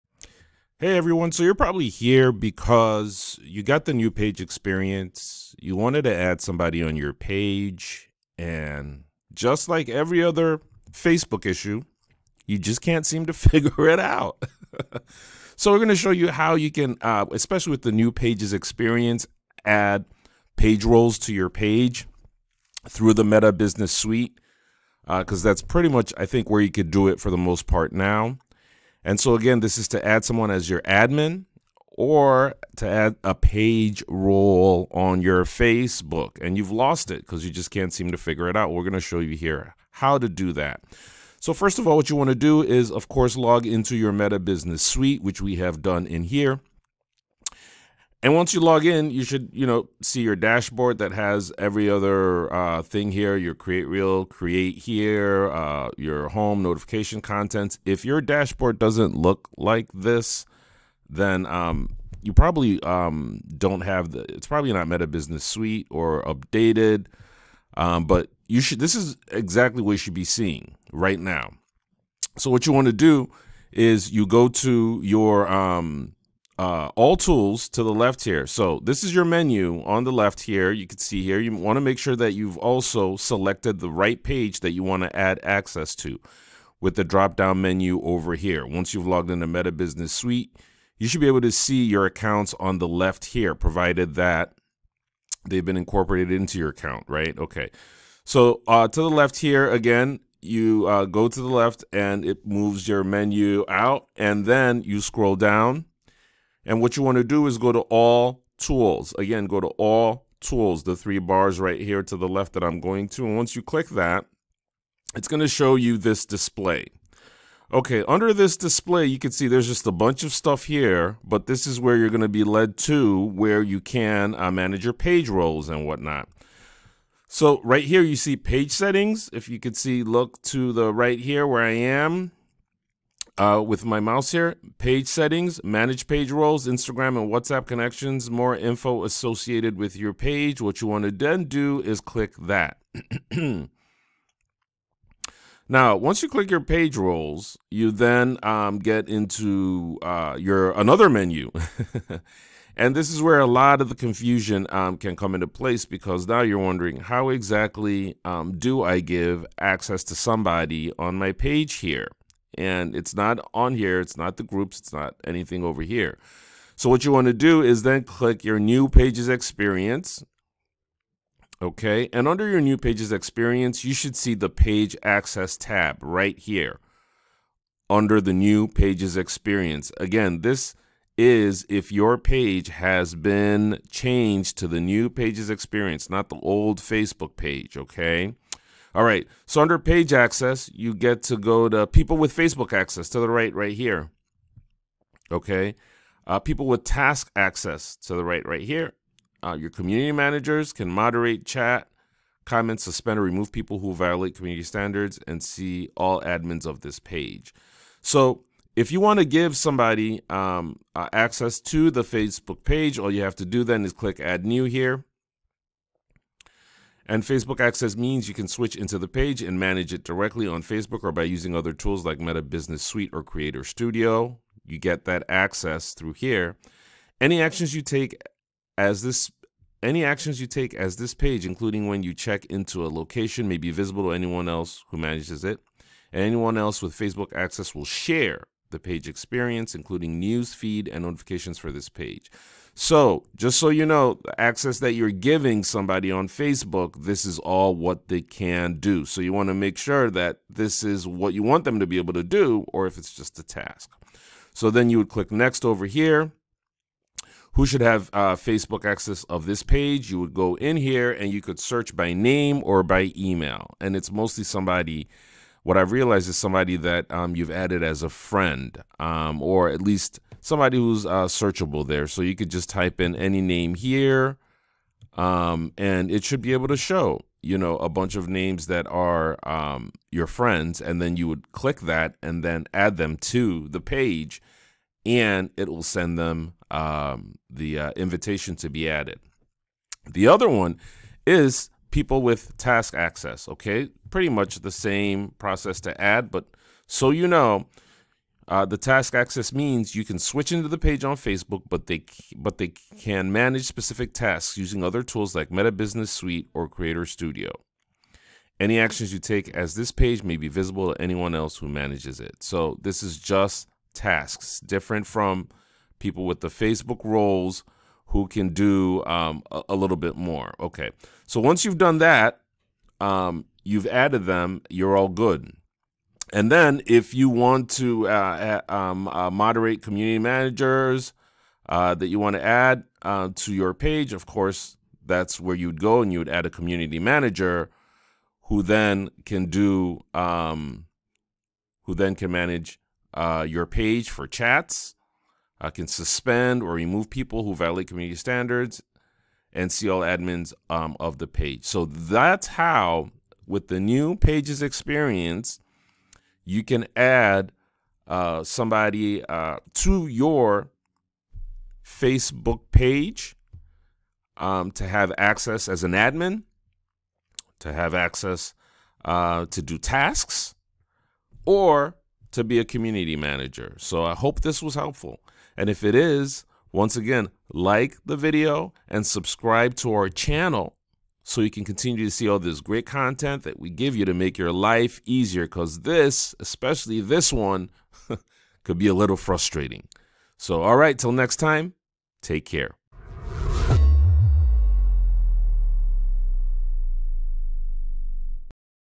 Can't Find Add Admin on Facebook? Here's the New Way!
In this video, we'll show you how to add an admin to your Facebook page.
This is a great way to give someone else access to your page and help manage it if you've updated to the New Page Experience. We'll show you how to do it in the new Facebook page experience and what the different roles mean.